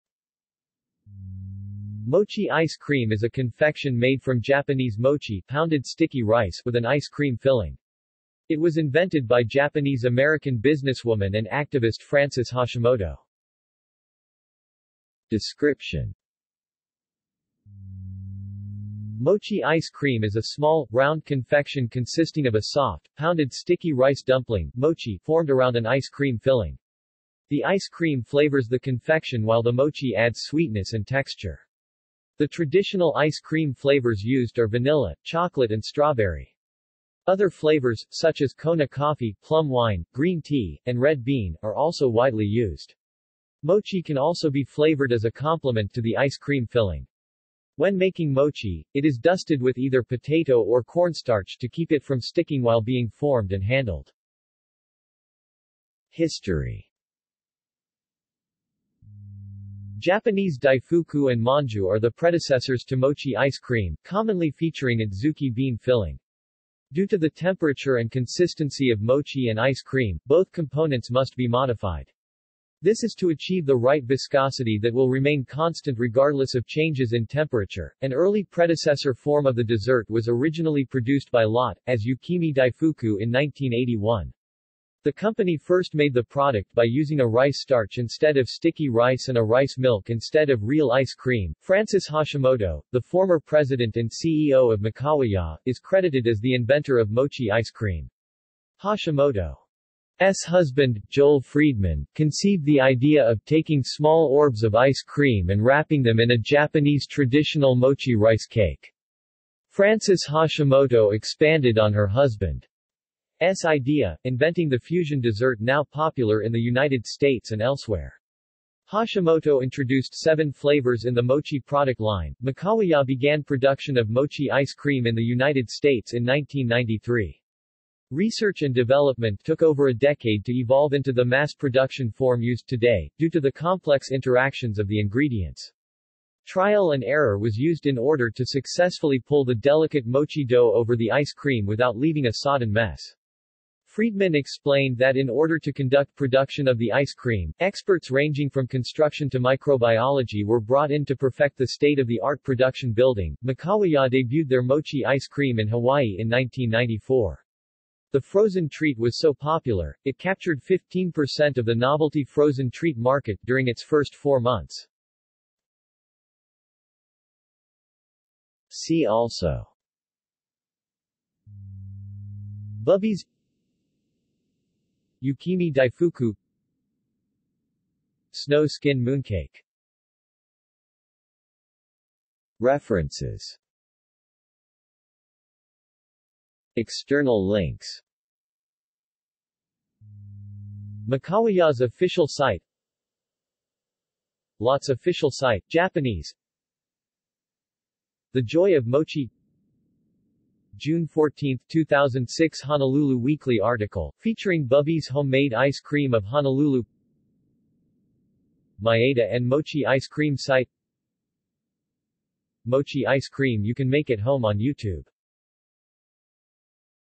Mochi ice cream | Wikipedia audio article
This is an audio version of the Wikipedia Article:
Mochi ice cream

SUMMARY
Mochi ice cream is a confection made from Japanese mochi (pounded sticky rice) with an ice cream filling. It was invented by Japanese-American businesswoman and activist Frances Hashimoto.